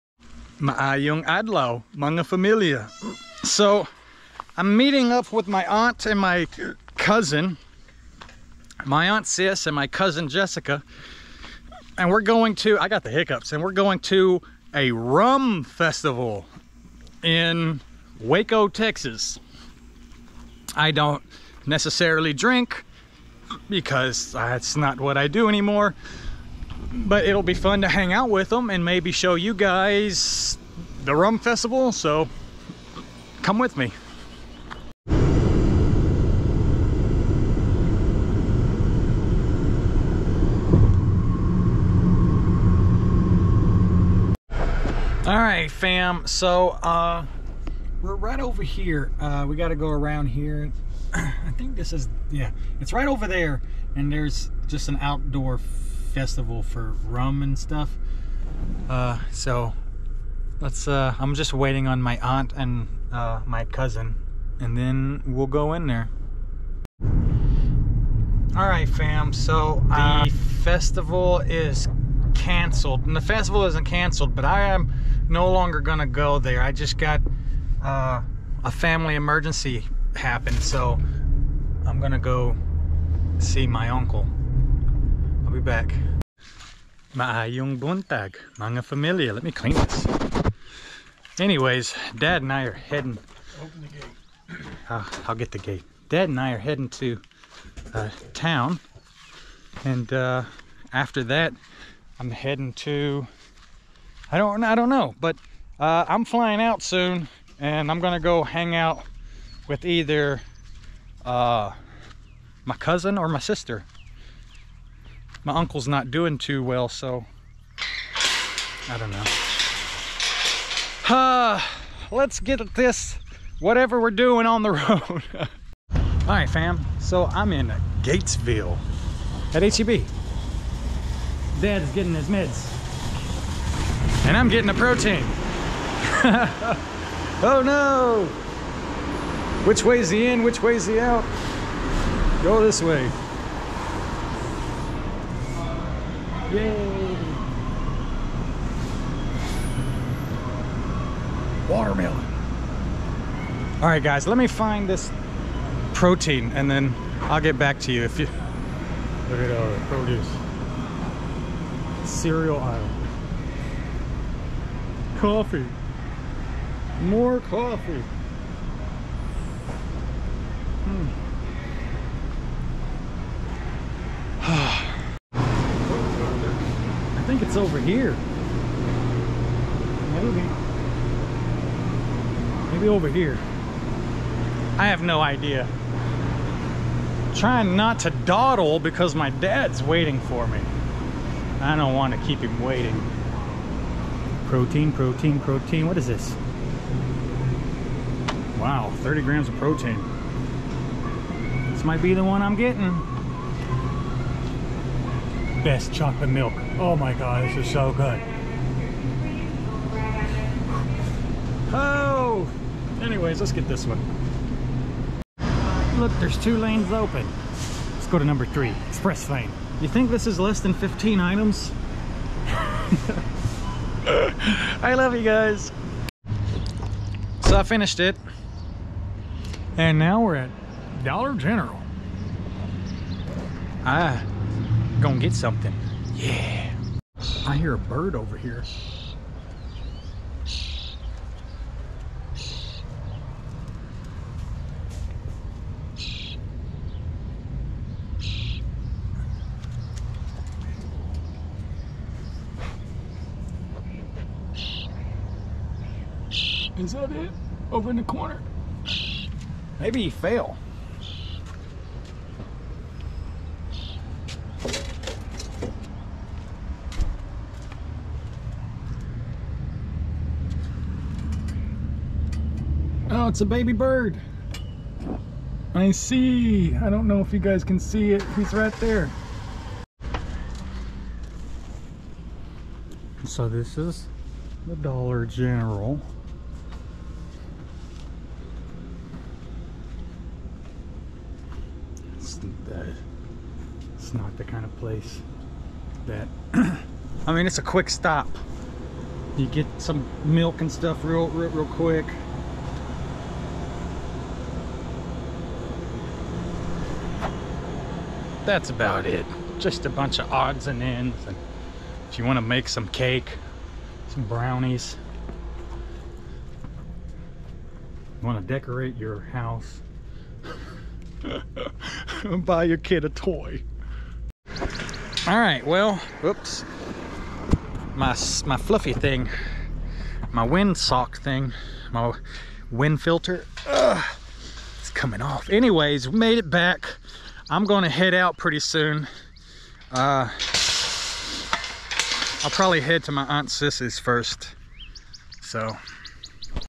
Rum festival? Last run to town, with my Dad | Crazy Kano is heading home soon
More content on this trip is available on my Facebook Page, A Crazy Kano in the Philippines...

Follow along as I live life in the Philippines